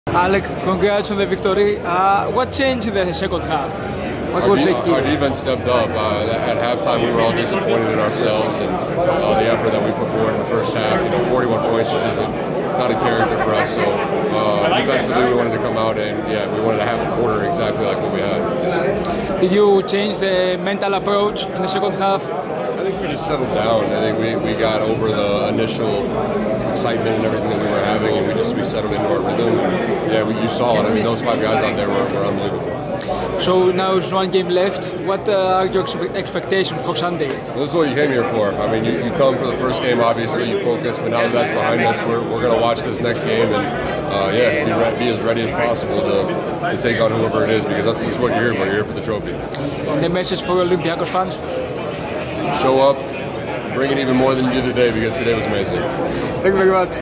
Alec Peters on Hedgeout.net about Olympiacos reaching euroleague Final, wild 3rd quarter run (27-2)
Alec Peters and Olympiacos erased a 12-point deficit againt Monaco (29-41) and advanced to the EuroLeague Championship Game, following their victory in the semi-final (78-62).

After the game, Alec Peters spoke with Hedgeout.net in the Mixed Zone about the team’s insane third quarter, how the team approached the game mentally in the second half and called more Olympiacos fans to travel to Kaunas for Sunday’s Final.

Alec Peters called the team’s third-quarter performance ‘unbelievable’, as Olympiacos made history with its 27-2 scoring run.